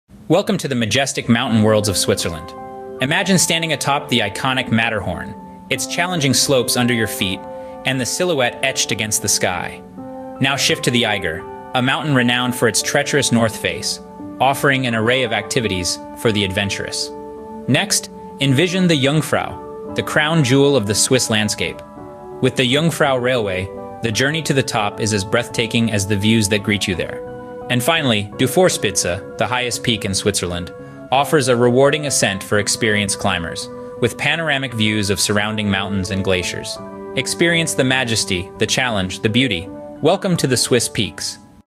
Switzerland's Majestic Peaks: A Quick Tour
In this captivating video, we take you on a virtual tour of Switzerland's top 11 mountains, showcasing their majestic beauty like never before. Inspired by a fascinating blog post, we've compiled fascinating facts, breathtaking footage, and useful tips that will leave you in awe of these stunning peaks.

From the iconic Matterhorn to the snow-capped beauty of Jungfrau, each mountain has a unique story to tell. Join us as we explore the awe-inspiring landscapes, the picturesque hiking trails, and the panoramic views that will take your breath away.

Whether you are an adventure enthusiast, a nature lover, or simply seeking some travel inspiration, this video is a must-watch. Get ready to immerse yourself in Switzerland's natural wonders, feel the adrenaline rush, and experience the serenity of the mountains.